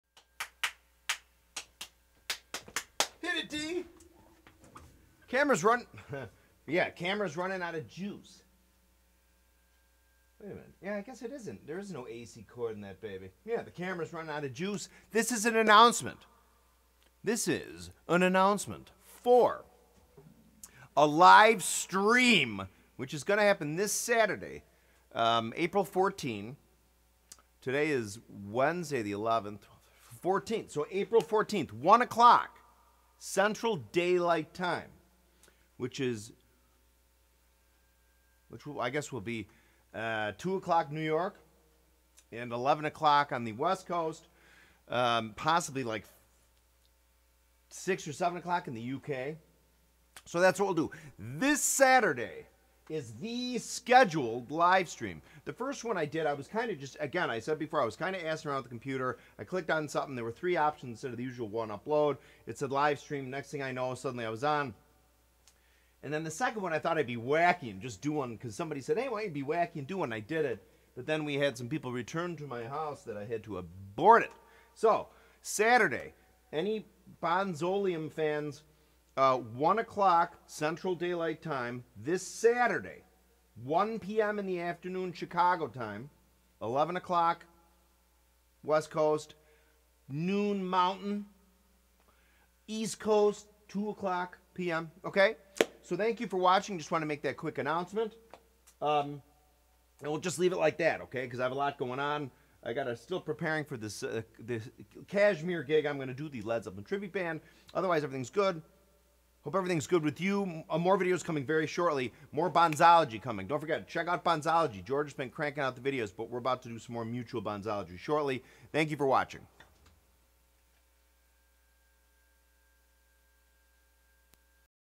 Announcement * Bonzoleum LIVE STREAM THIS SATURDAY April 14 At 1:00pm CDT *
This is the official announcement of the YouTube live stream I will do this Saturday (April 14 2018) at 1 pm CDT here on my channel. That will put it in the later afternoon/early evening on "The Continent". With the Live Stream you can comment & ask questions in real time. It will be LIVE.
Thank you sincerely-very sincerely-to my fans. I really appreciate your fandom.

9:55